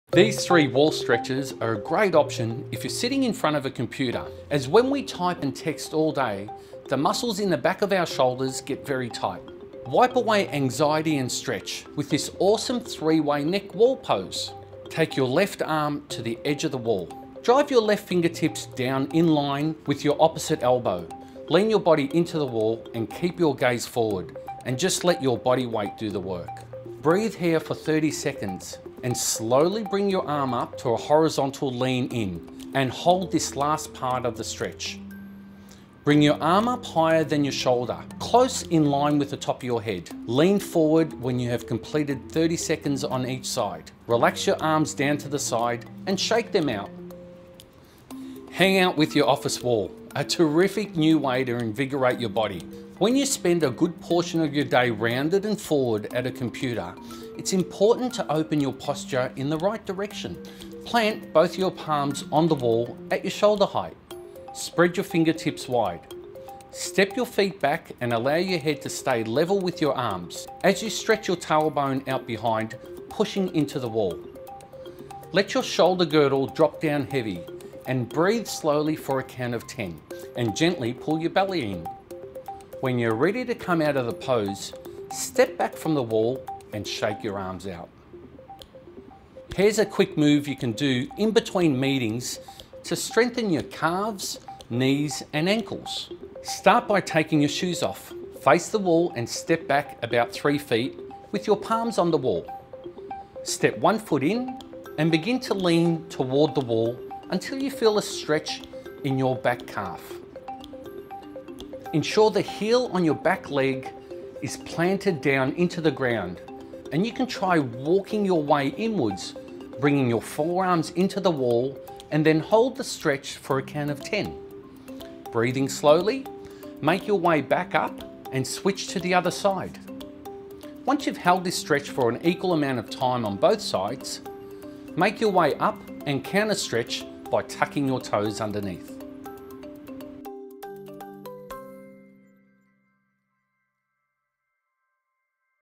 3 great wall Stretches
these 3 wall stretches are a great option when sitting in front of a computer this awesome 3 way wall pose will make you feel better and give you a real great feeling